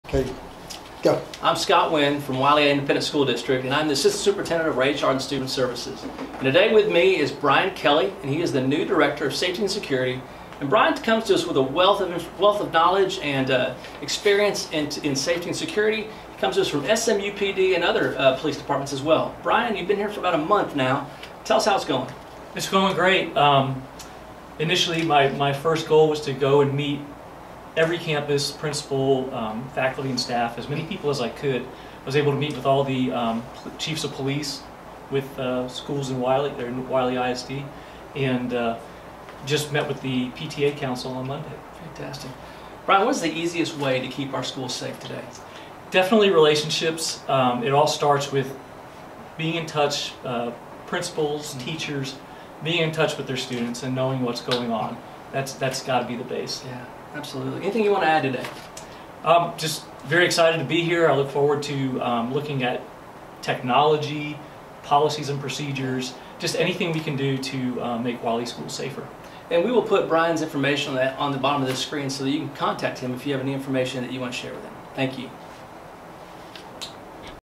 Safety and Security Minute with Mr. Brian Kelly
Mr. Brian Kelly is the Director of Safety and Security for the Wylie Independent School District. In this video, Assistant Superintendent for Student Services and Human Resources welcomes Mr. Kelly and together, we are asking for suggestions for future topics of discussion. Thanks in advance for helping to keep our kids safe, the Wylie Way!